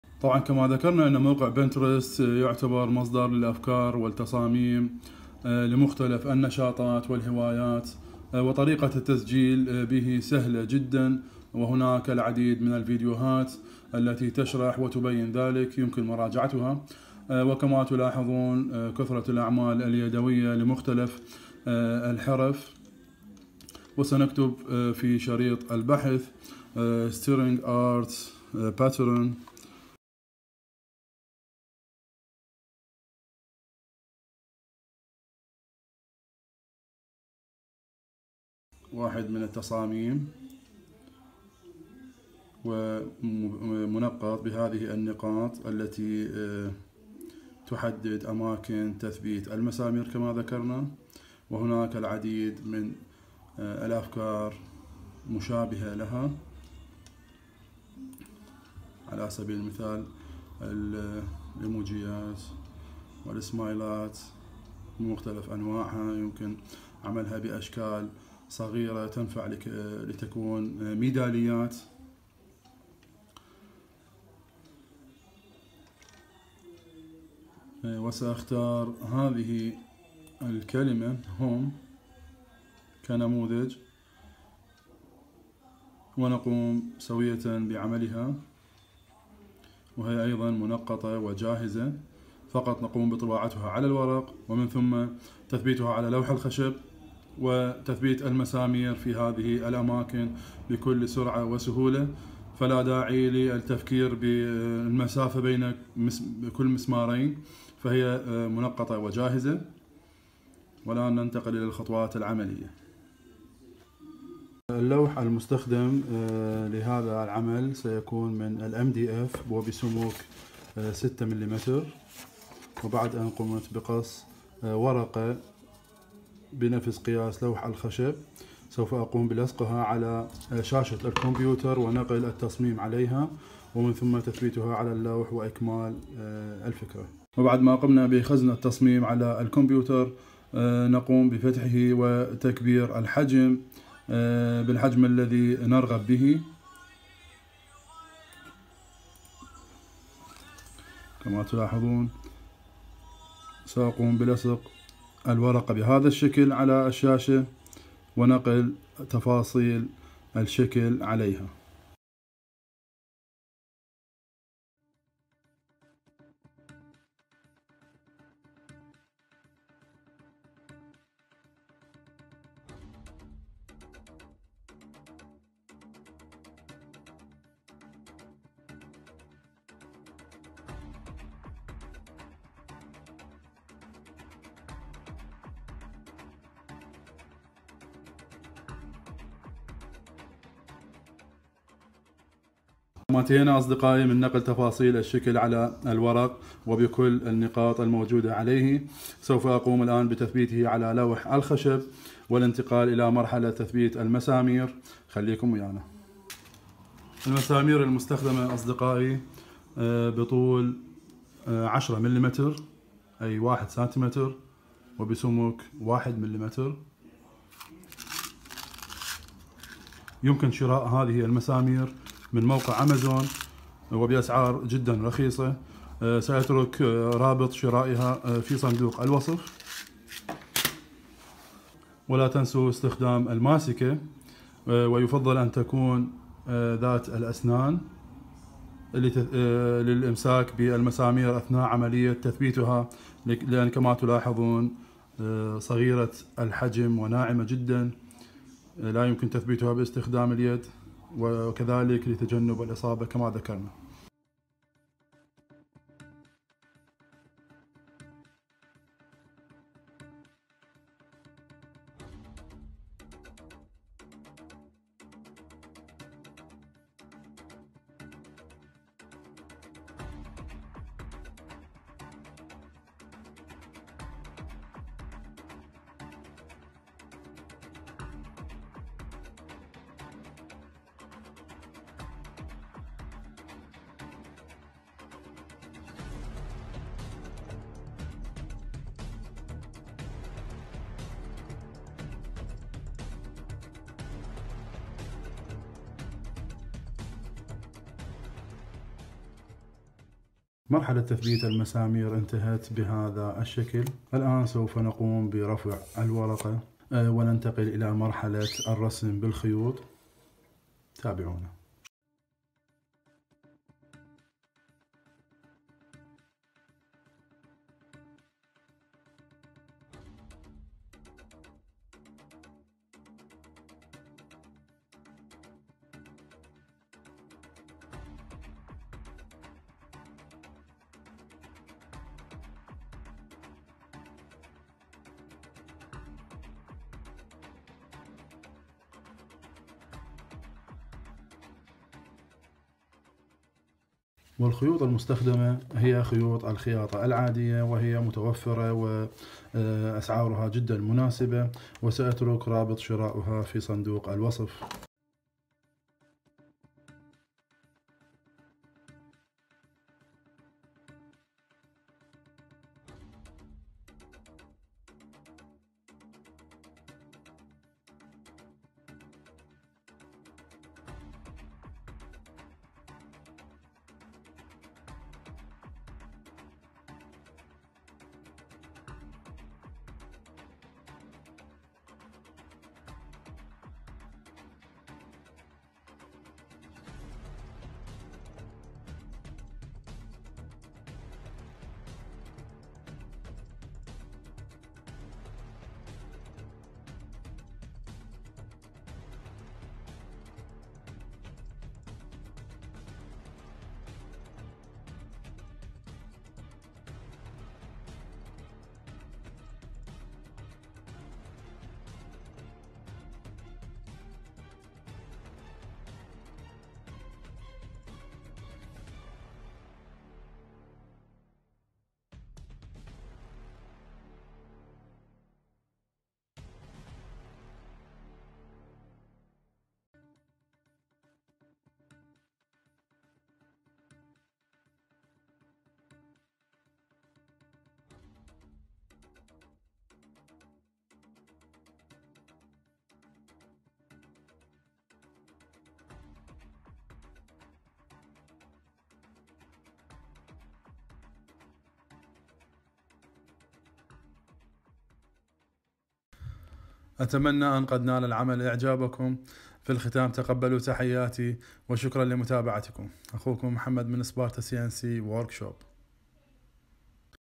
الرسم بالخيوط والمسامير (تصاميم منقطة جاهزة)
من ضمن تدريبات
طريقة تنزيل تصاميم منقطة وجاهزة بتأشيرات لاماكن تثبيت المسامير
رابط شراء المسامير
رابط شراء الخيوط
اذا اعجبتكم قناتي يمكنكم دعمها ماديا لتتطور وتستمر على الباتريون على الرابط ادناه